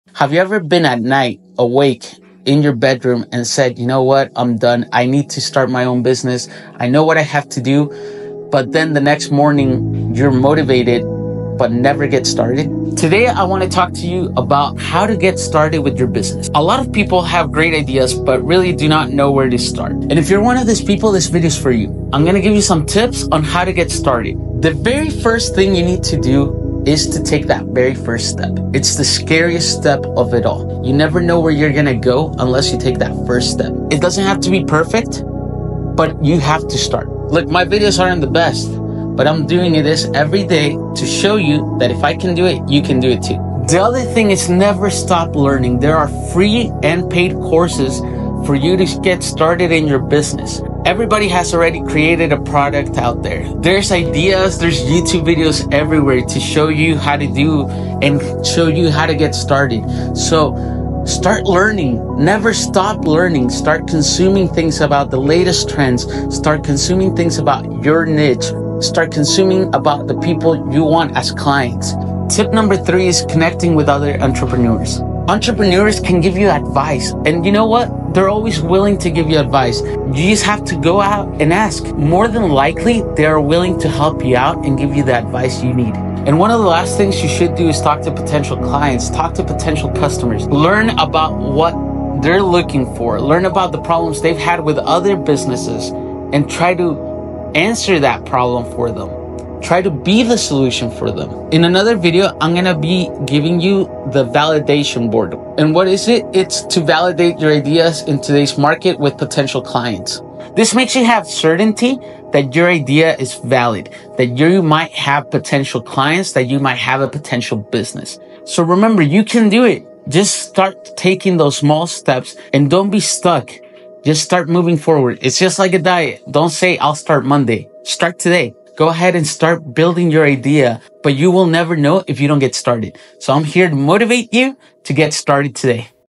Get Started Tips for smll businesses
💼💡 Shed your fear and take control of your entrepreneurial destiny! I share some key tips for starting your own business.